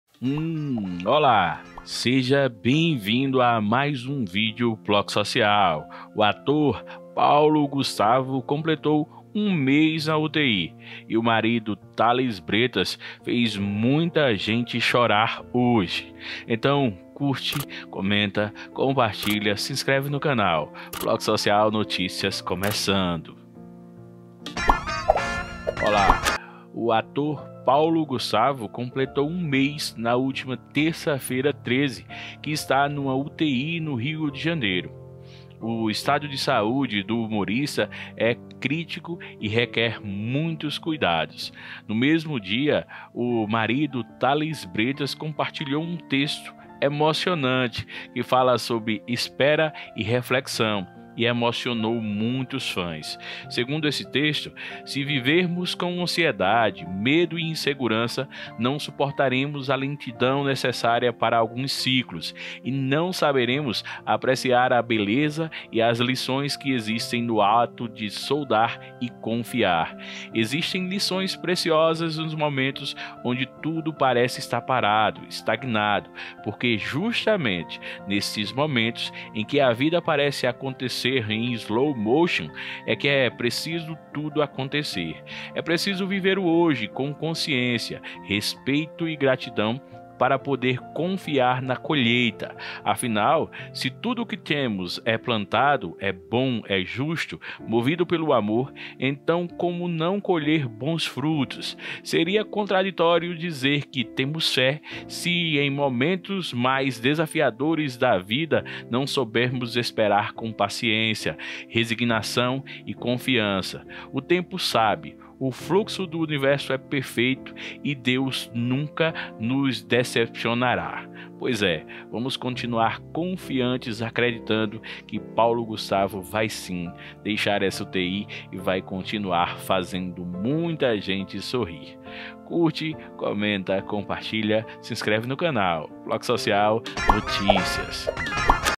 EMOCIONANTE, APÓS CIRURGIAS E ESTADO GRAVE, PAULO GUSTAVO SEGUE EM ESTABILIDADE
Com Paulo Gustavo internado há um mês para tratar complicações da Covid-19, familiares e amigos têm se unido em correntes de oração. Confiantes de que o ator vai superar esta doença, a ansiedade para vê-lo bem logo costuma bater.